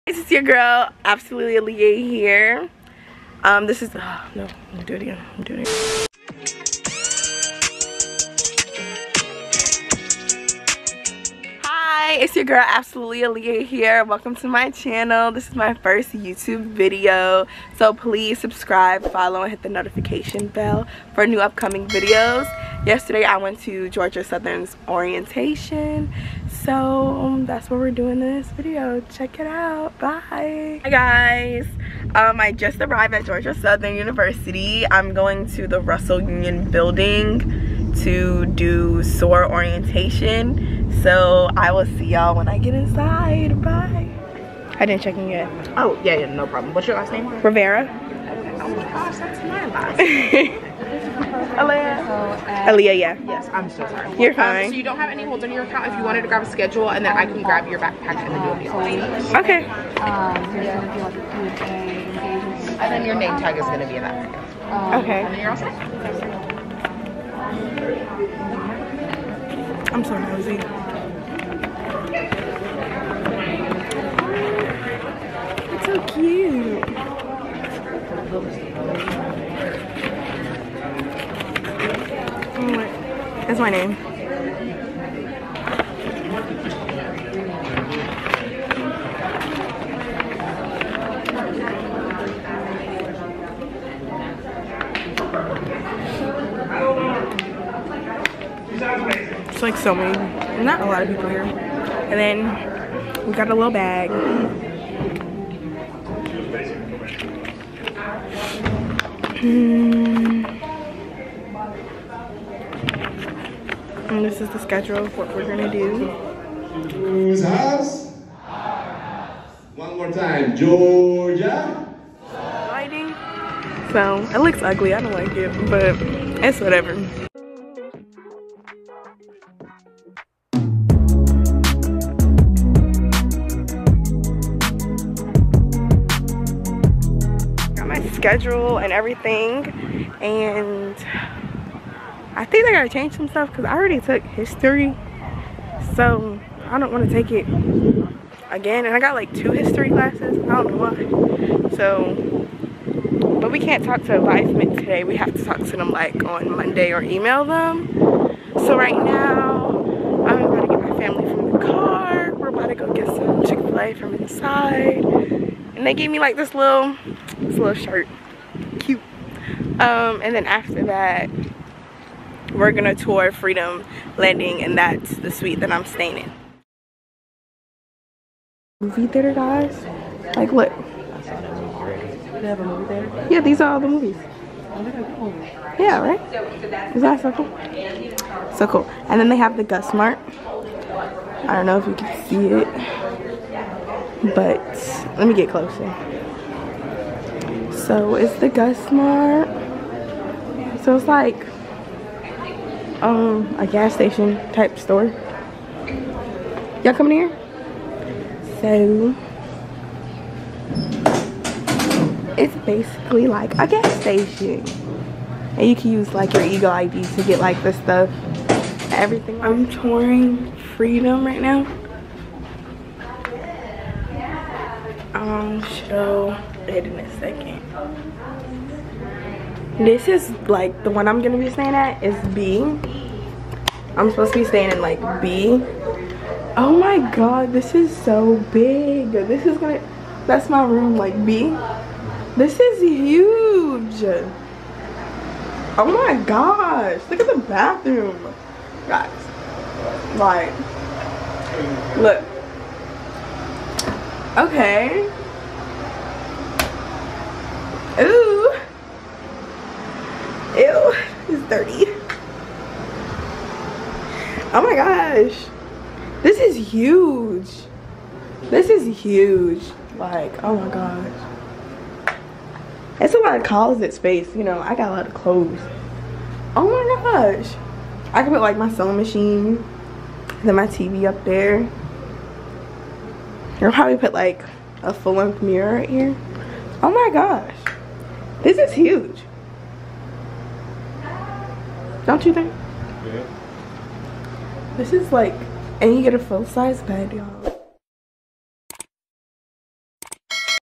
Georgia Southern Orientation (Soar)
I Know My Future Eagles might have questions or worry about SOAR Orientation. Hopefully this gives you a run down on what to expect!

Music 🎶